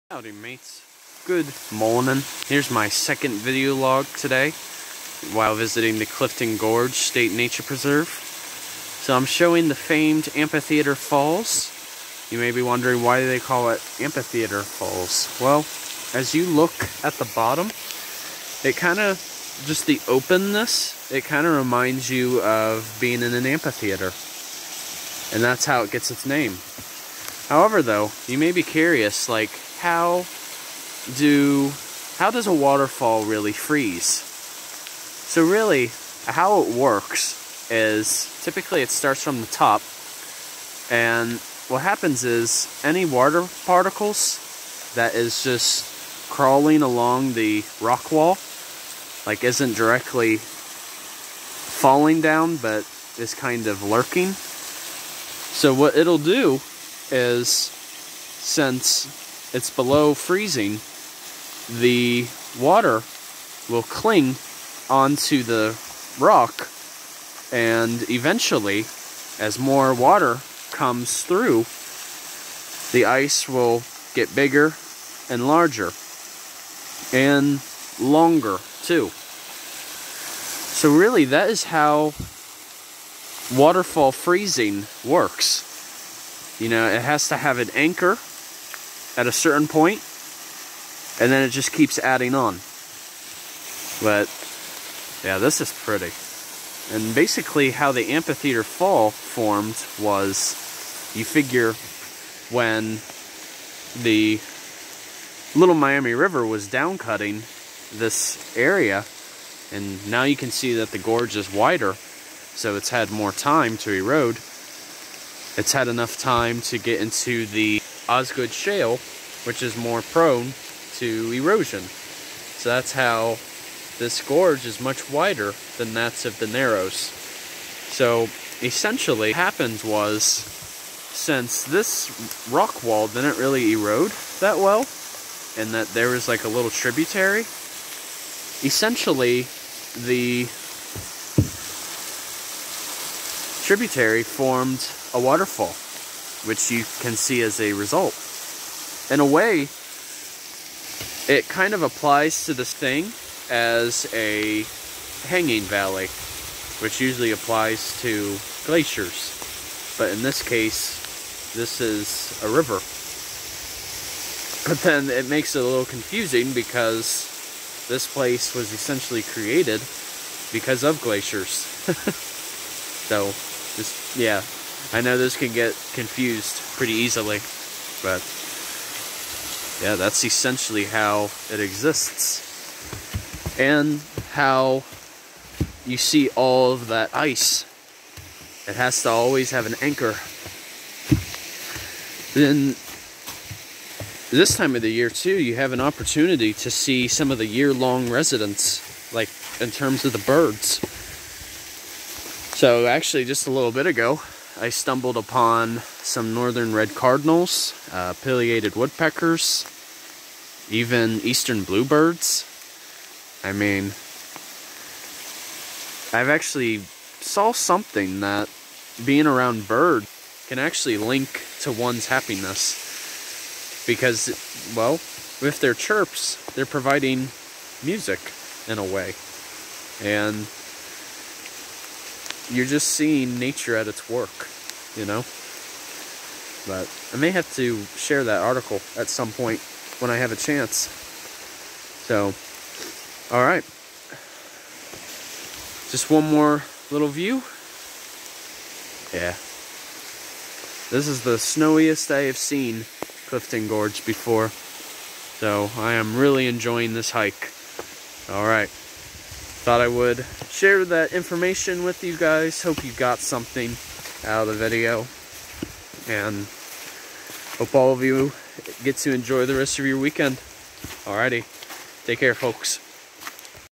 Amphitheater Falls
Video #2:
Amphitheater Falls! I discuss how the ice formations occur and I describe the rock strata that you can see.
Enjoy mates 🤙